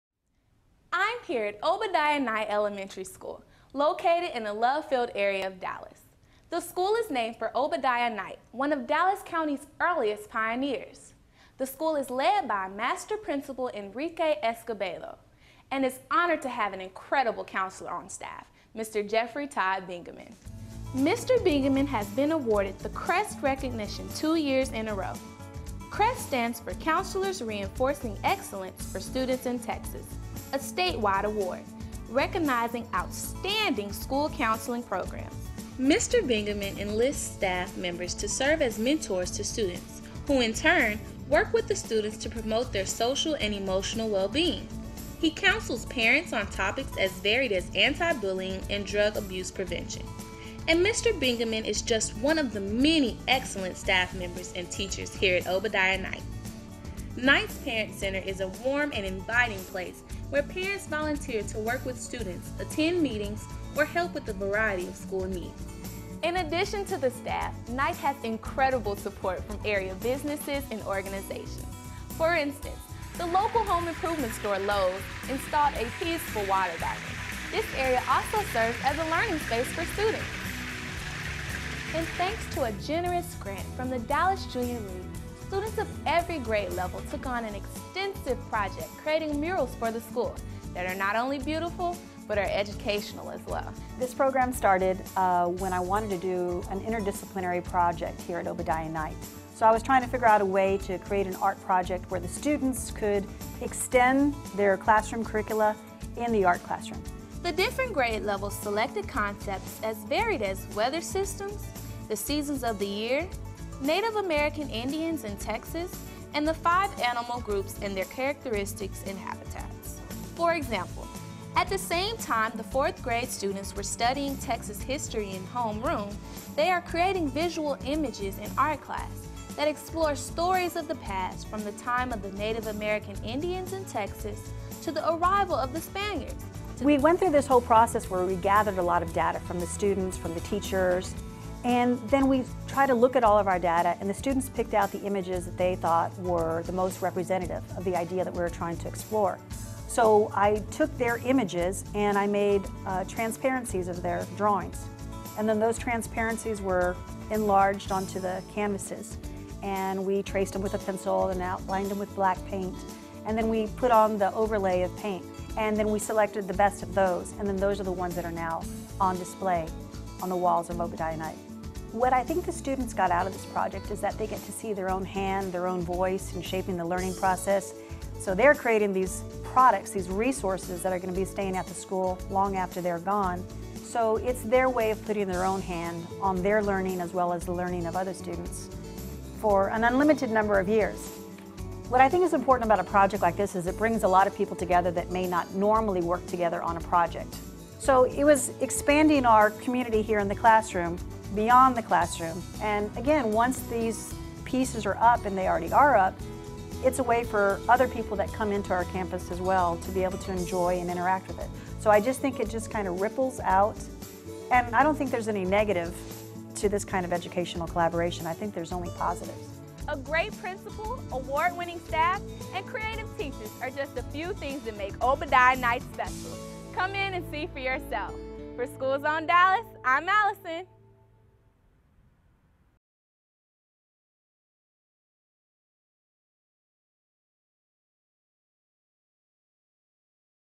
Dallas ISD: Obadiah Knight Elementary School Promo
I'm here at Obadiah Knight Elementary School located in the Love Field area of Dallas, the school is named for Obadiah Knight, one of Dallas County's earliest pioneers.

The school's principal, Enrique Escobedo, has outstanding leadership skills and has led Knight to numerous academic awards. Also, Knight is honored to have an incredible counselor on staff, Mr. Jeffery Todd Bingamon.

Mr. Bingamon has been awarded the CREST recognition two years in a row. CREST stands for "Counselors Reinforcing Excellence for Students in Texas" a statewide award, recognizing outstanding school counseling programs. Mr. Bingamon enlists staff members to serve as mentors to students, who in turn, work with the students to promote their social and emotional well-being. He counsels parents on topics as varied as anti-bullying, and drug abuse prevention. And Mr. Bingamon is just one of the many excellent staff members and teachers here at Obadiah Knight.